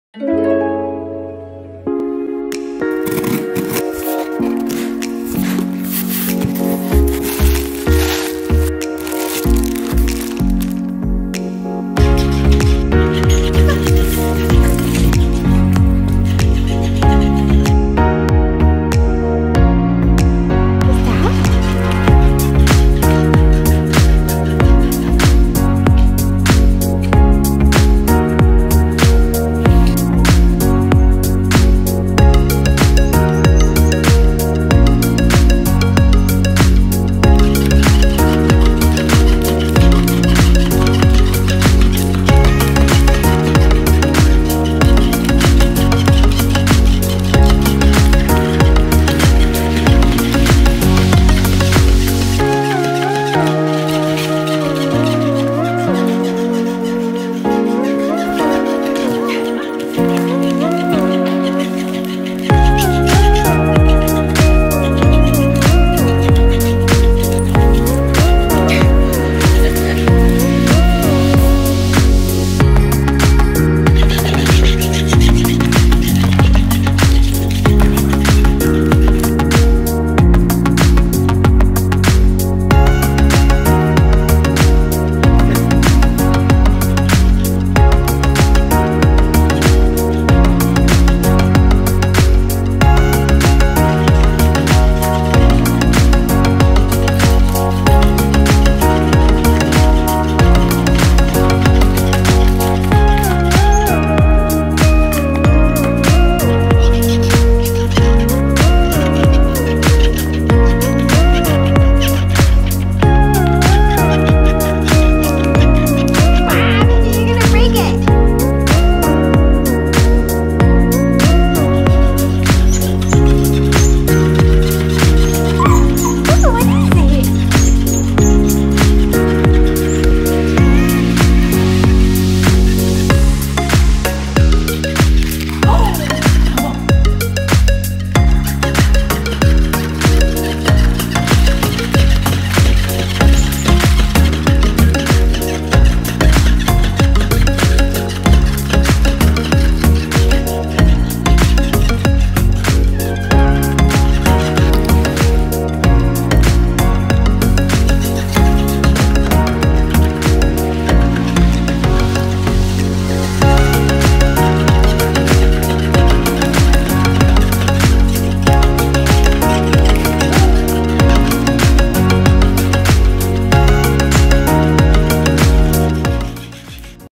Can Dogs Eat Fish? | Dogs vs Fish - Inspired by Kittisaurus
Inspired by Cats vs Fish | Kittisaurus video. We do a dogs vs fish video! Can dogs eat fish? In this video, Mitchy the Miniature Poodle and Tofu the Samoyed get a fun surprise! We ordered what was supposed to be a cat toy from Amazon! We have seen fellow youtubers get this toy for their cats and dogs to play with and we just had to get it for Mitchy and Tofu to test it out too! It’s a fish that moves when it’s slapped or touched and will also stop moving on its own when the dogs stop playing with it. It was so fun to see their reactions for the first time to a toy that moves on its own!